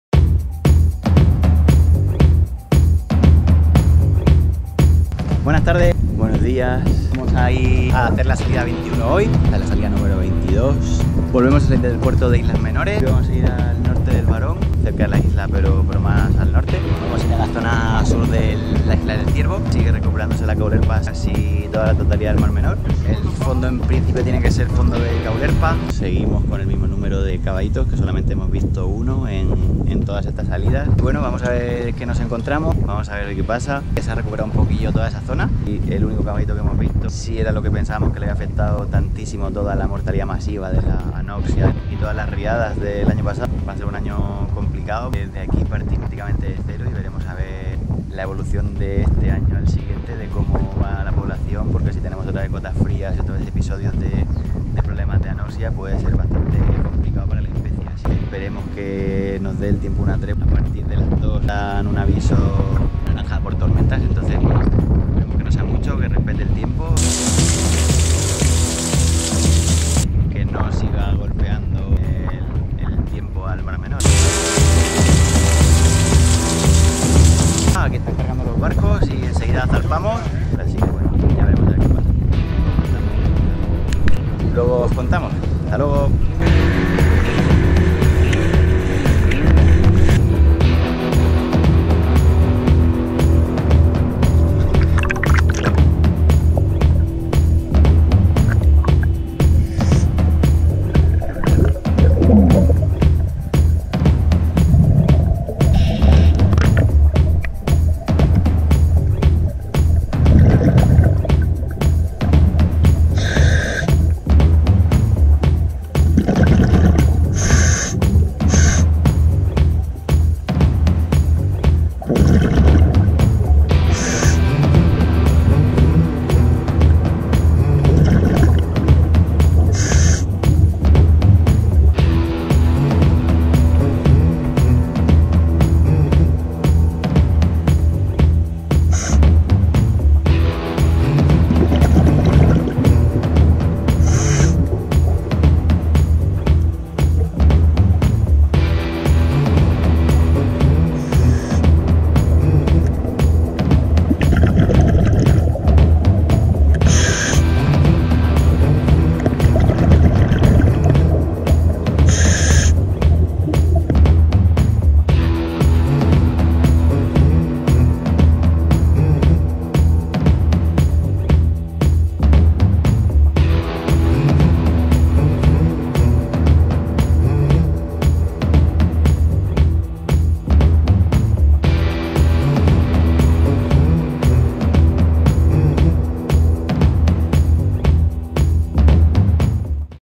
Asociación Hippocampus, 21ª y 22ª SALIDA CUBETA CENTRAL del Mar Menor, (26 y 29 de agosto de 2020)
Inmersión de buceo por voluntarios de la Asociación Hippocampus en la laguna del Mar Menor en lña Región de Murcia, en busca de Caballitos de Mar, de la especie Hippocampus guttulatus, para determinar el estado de su población y poder así solicitar que se eleve a una posición jurídica sólida para su protección y recuperación.